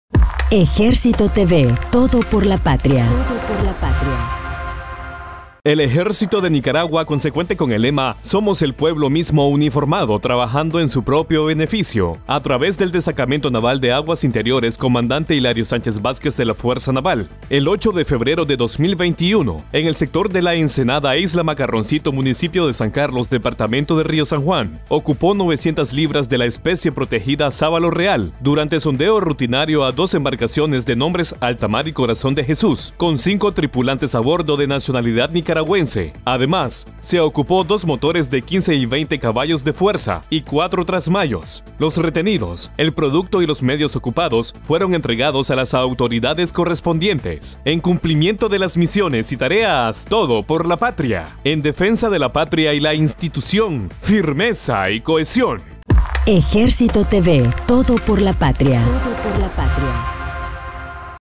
Retención de personas por pesca ilegal y ocupación de 900 lb de especies protegidas
El Ejército de Nicaragua a través del Destacamento Naval de Aguas Interiores “Cmte. Hilario Sánchez Vásquez” de la Fuerza Naval, el 8 de febrero de 2021, en el sector de La Ensenada, Isla Macarroncito, municipio de San Carlos, departamento de Río San Juan, ocupó 900 lb de la especie protegida Sábalo Real.